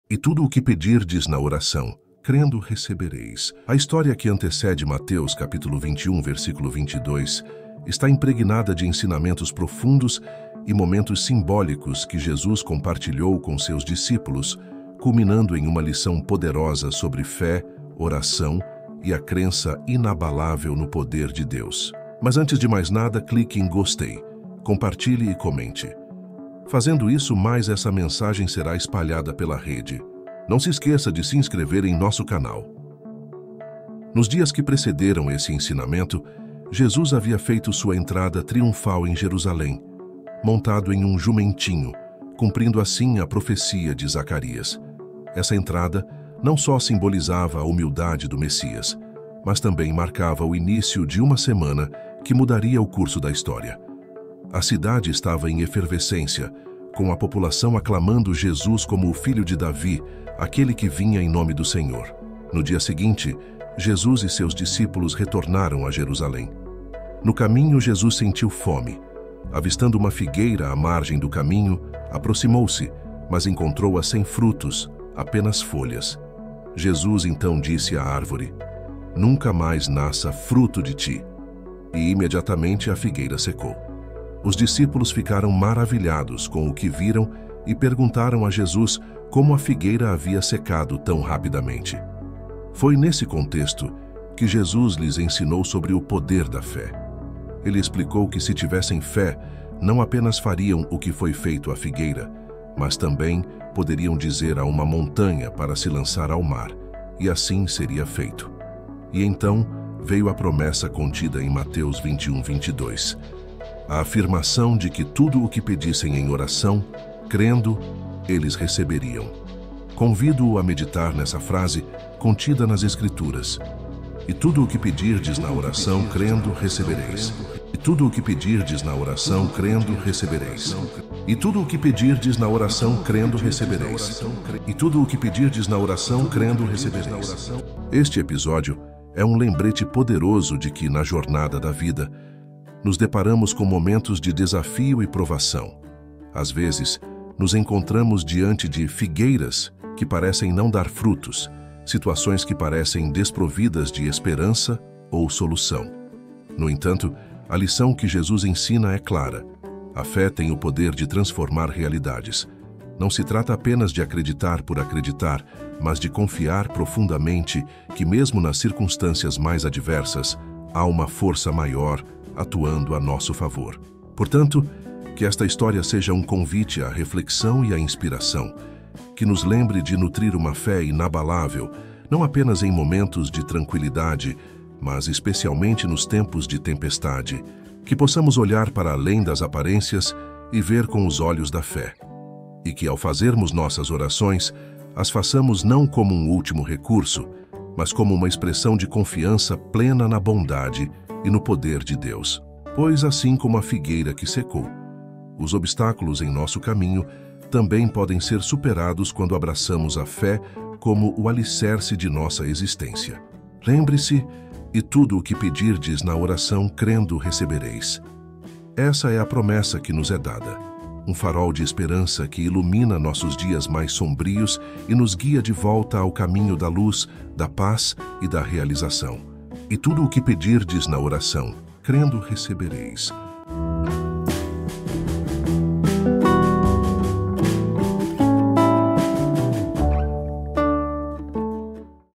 O poder da mente na Bíblia
00:00 Mateus 21:22 - "E tudo o que pedirdes na oração, crendo, recebereis."

03:16 A história que antecede Mateus 21:22 está impregnada de ensinamentos profundos e momentos simbólicos que Jesus compartilhou com seus discípulos, culminando em uma lição poderosa sobre fé, oração e a crença inabalável no poder de Deus.

00:33 Nos dias que precederam esse ensinamento, Jesus havia feito sua entrada triunfal em Jerusalém, montado em um jumentinho, cumprindo assim a profecia de Zacarias. Essa entrada não só simbolizava a humildade do Messias, mas também marcava o início de uma semana que mudaria o curso da história. A cidade estava em efervescência, com a população aclamando Jesus como o Filho de Davi, aquele que vinha em nome do Senhor.

01:02 No dia seguinte, Jesus e seus discípulos retornaram a Jerusalém. No caminho, Jesus sentiu fome. Avistando uma figueira à margem do caminho, aproximou-se, mas encontrou-a sem frutos, apenas folhas. Jesus então disse à árvore: "Nunca mais nasça fruto de ti!" E imediatamente, a figueira secou. Os discípulos ficaram maravilhados com o que viram e perguntaram a Jesus como a figueira havia secado tão rapidamente.

01:32 Foi nesse contexto que Jesus lhes ensinou sobre o poder da fé. Ele explicou que se tivessem fé, não apenas fariam o que foi feito à figueira, mas também poderiam dizer a uma montanha para se lançar ao mar, e assim seria feito. E então veio a promessa contida em Mateus 21:22: a afirmação de que tudo o que pedissem em oração, crendo, eles receberiam.
Convido-o a meditar nessa frase, contida nas Escrituras:
"E tudo o que pedirdes na oração, crendo, recebereis."

02:17 Este episódio é um lembrete poderoso de que, na jornada da vida, nos deparamos com momentos de desafio e provação. Às vezes, nos encontramos diante de "figueiras" que parecem não dar frutos, situações que parecem desprovidas de esperança ou solução. No entanto, a lição que Jesus ensina é clara: a fé tem o poder de transformar realidades. Não se trata apenas de acreditar por acreditar, mas de confiar profundamente que, mesmo nas circunstâncias mais adversas, há uma força maior atuando a nosso favor.

02:55 Portanto, que esta história seja um convite à reflexão e à inspiração. Que nos lembre de nutrir uma fé inabalável, não apenas em momentos de tranquilidade, mas especialmente nos tempos de tempestade.

03:05 Que possamos olhar para além das aparências e ver com os olhos da fé. E que, ao fazermos nossas orações, as façamos não como um último recurso, mas como uma expressão de confiança plena na bondade e no poder de Deus.
Pois, assim como a figueira que secou, os obstáculos em nosso caminho também podem ser superados quando abraçamos a fé como o alicerce de nossa existência.

03:33 Lembre-se: "E tudo o que pedirdes na oração, crendo, recebereis." Essa é a promessa que nos é dada, um farol de esperança que ilumina nossos dias mais sombrios e nos guia de volta ao caminho da luz, da paz e da realização.